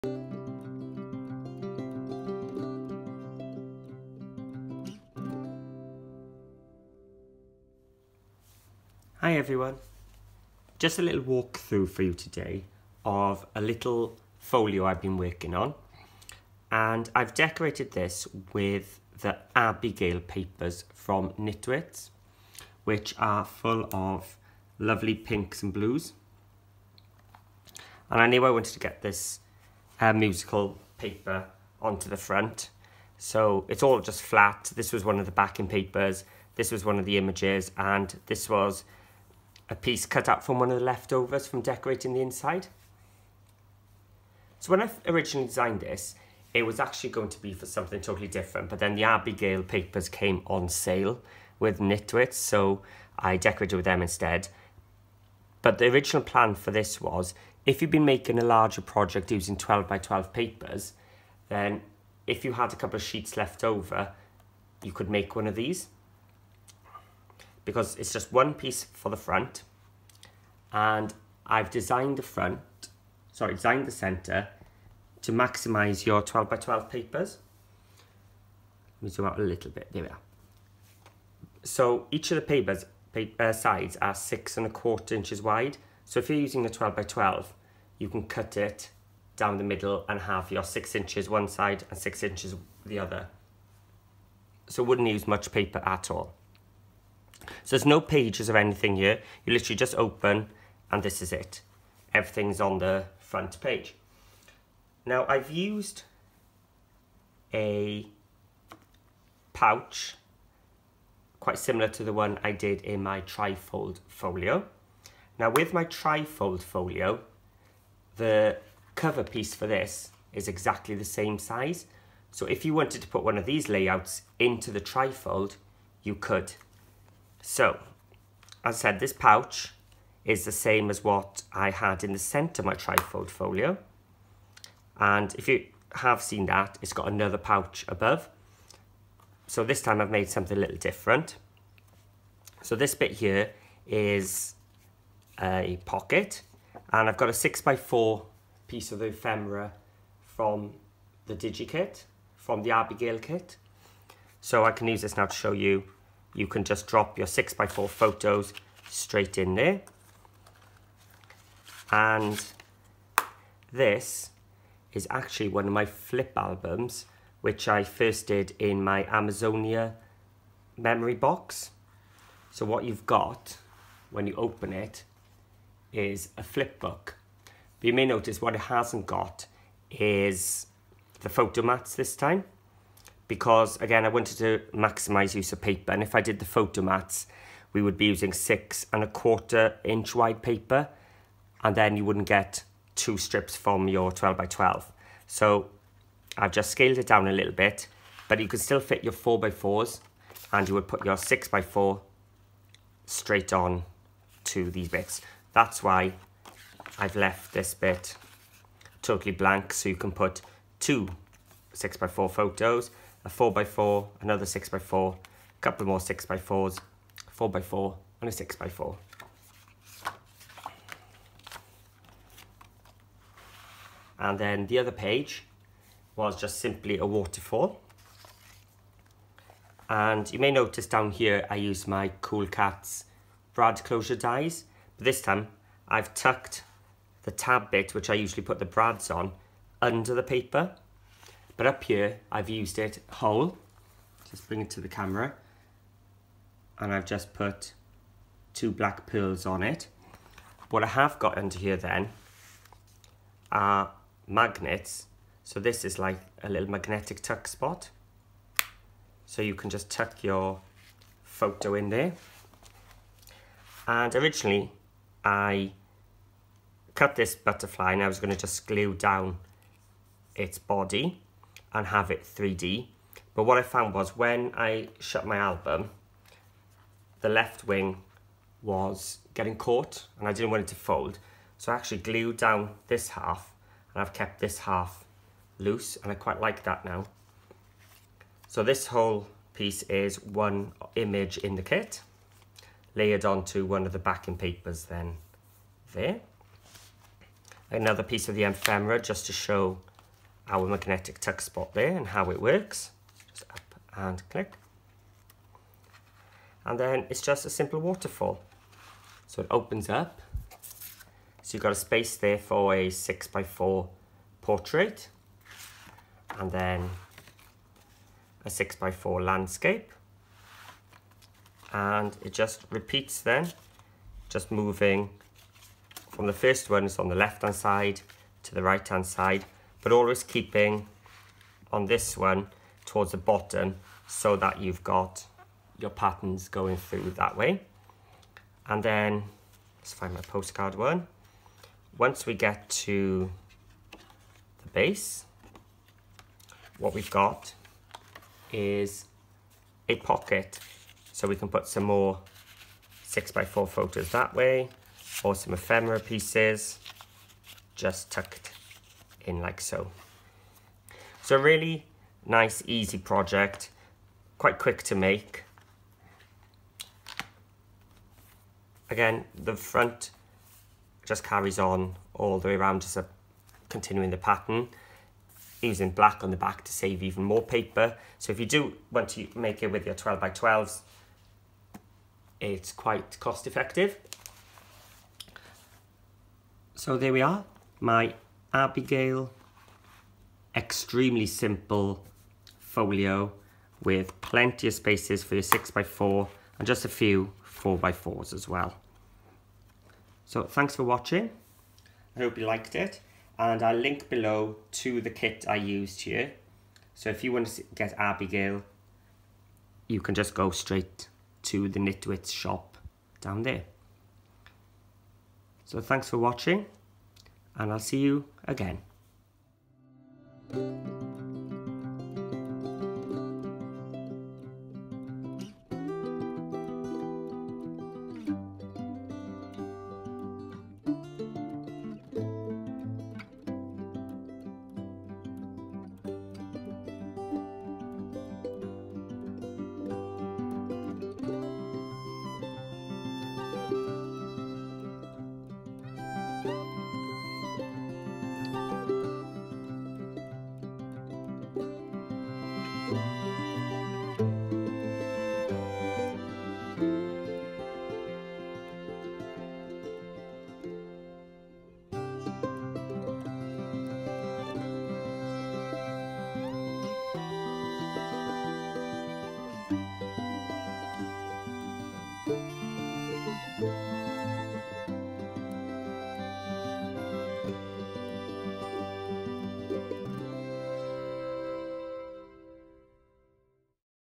The Abigail Really Simple Folio Album
This is a REALLY simple folio. No pages, just using the inside covers yet it still has places for a whole host of photos. I used the Abigail papers to decorate it which can be bought and downloaded from Nitwit Collections: